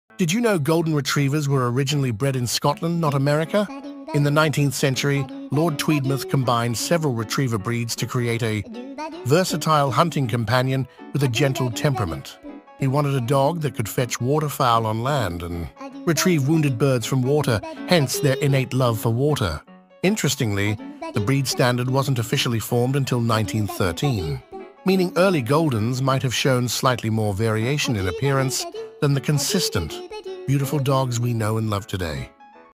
SCOTLAND'S GOLDEN GIFT: Unveiling the Fascinating Past of the Golden Retriever
Did you know these beloved companions originated in Scotland, not America? Lord Tweedmouth meticulously bred them in the 19th century for their versatility and gentle nature. He sought a companion adept at both land and water retrieves, explaining their natural love for swimming. Interestingly, the official breed standard was only established in 1913, suggesting early Goldens displayed more diverse appearances than the uniform beauty we cherish today.